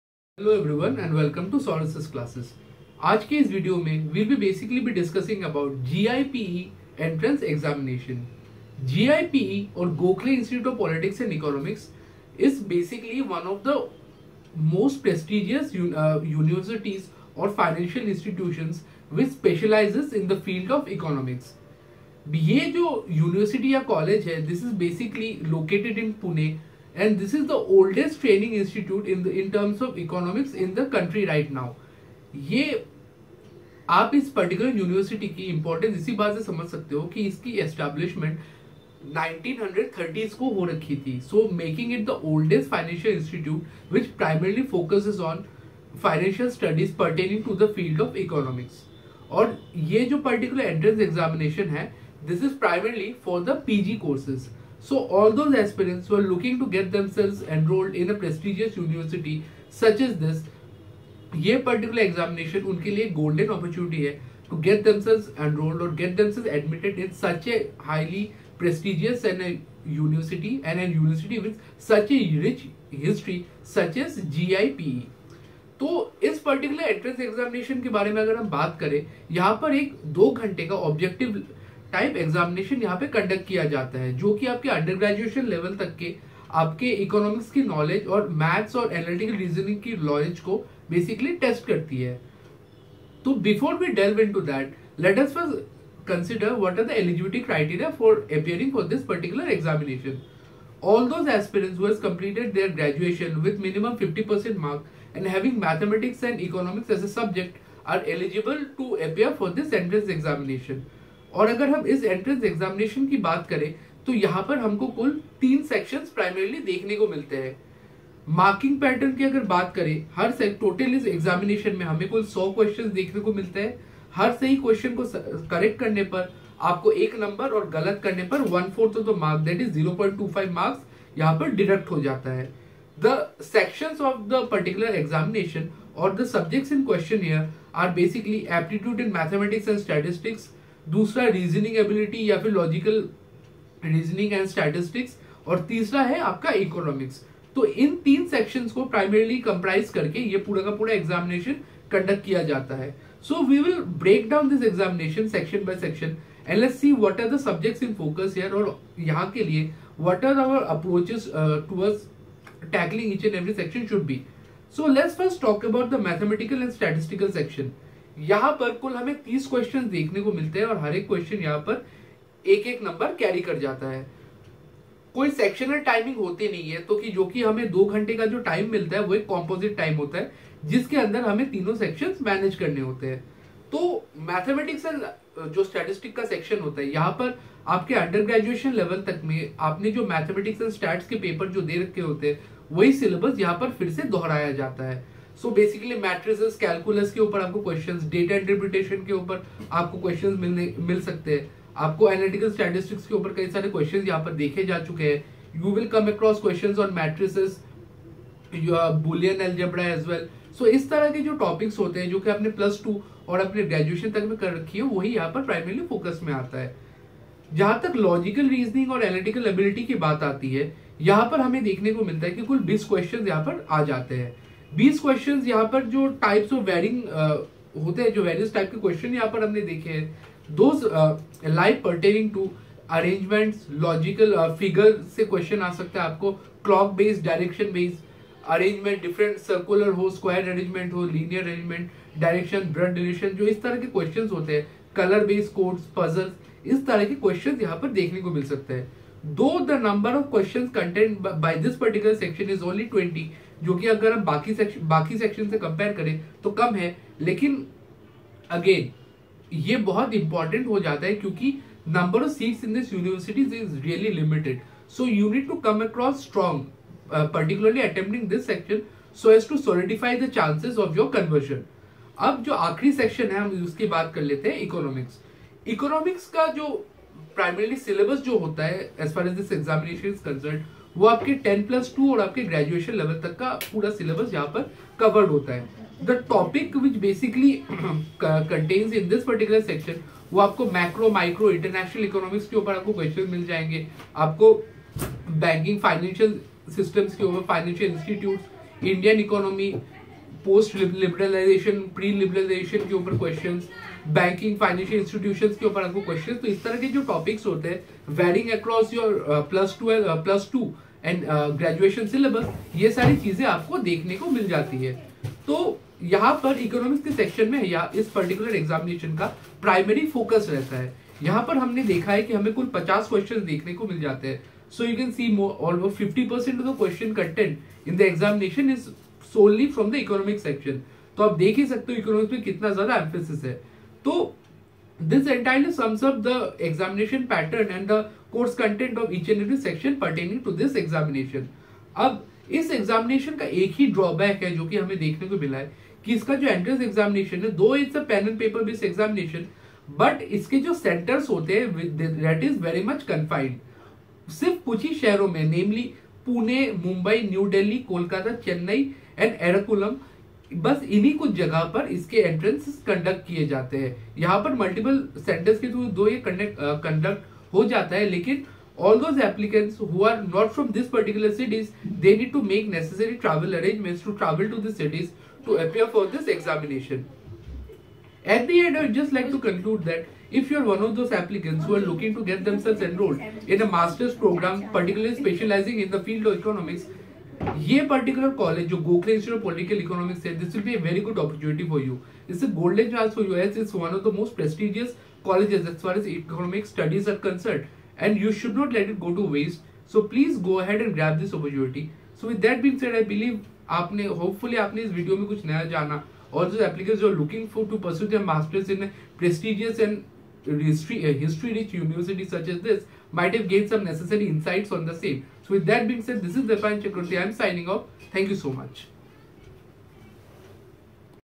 GOKHALE INSTITUTE OF POLITICS AND ECONOMICS(GIPE) DETAILED INFORMATION AND ANALYSIS WITH EXPLANATION
# GIPE # Information in Details # Full Information and Analysis # New Pattern # Exam Pattern # Economics

Gokhale Institute of Politics and Economics, commonly known as Gokhale Institute, is one of the oldest research and training institutes in Economics in India.

The Institute was founded on 6 June 1930 by R. R. Kale as a centre for research and higher learning in economics. The Institute was founded with an endowment offered to the Servants of India Society by Shri R R Kale. The Servants of India Society, a registered body founded by the nationalist leader Gopal Krishna Gokhale, are the trustees of the Institute. The Institute is registered under the Societies Registration Act, 1860, and the Bombay Public Trusts Act, 1950.

The Institute was set up with an objective to conduct research on the economic and political problems of India and to train research workers in these disciplines. D. R. Gadgil was the first Director of the Institute.